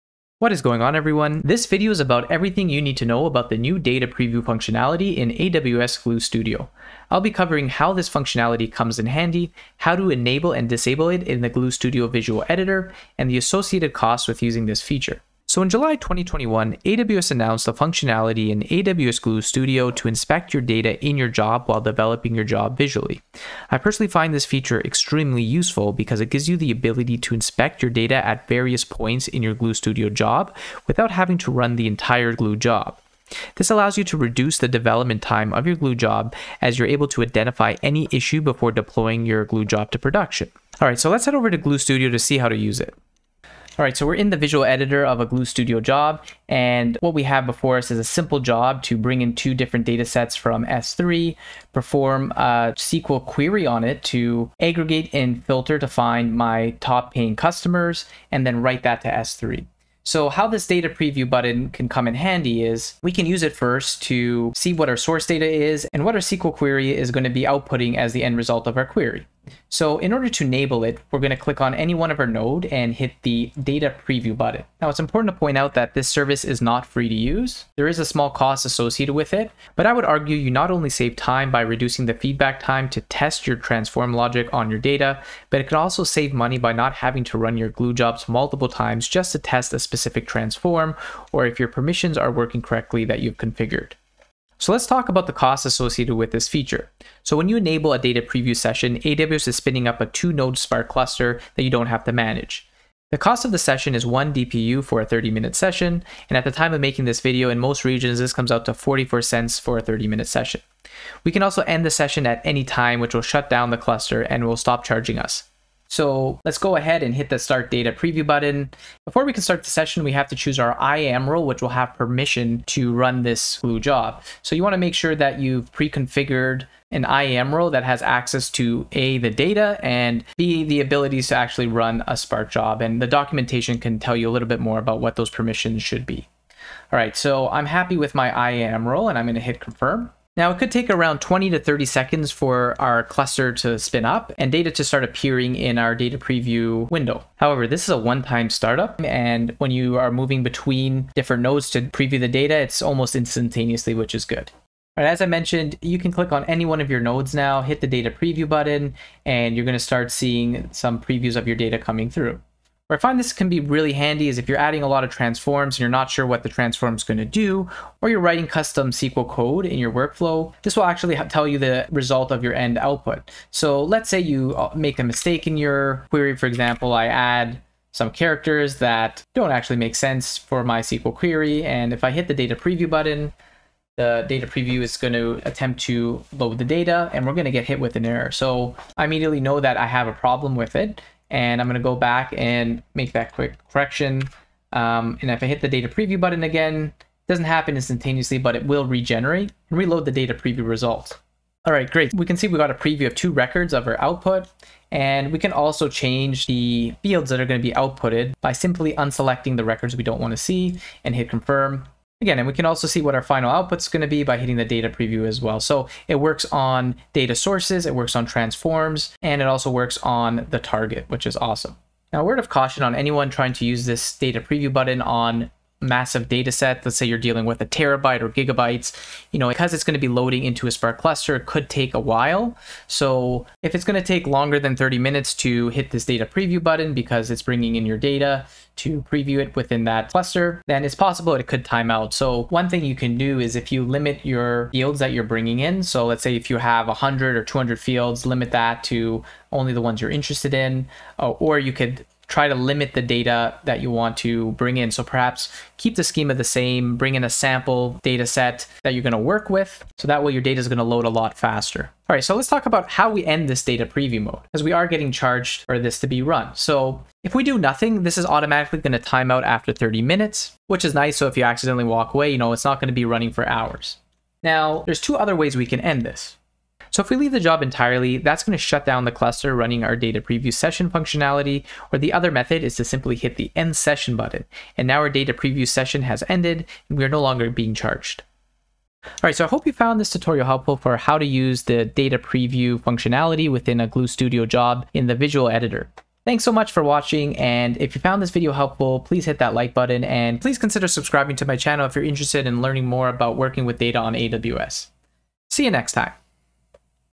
AWS Glue Studio: Data Preview Feature
This video is everything you need to know on how to use the data preview functionality in a visual AWS glue studio job. It covers how to disable and enable the feature as well as the additional costs associated with this feature since it leverages compute in order to generate your data in the visual console.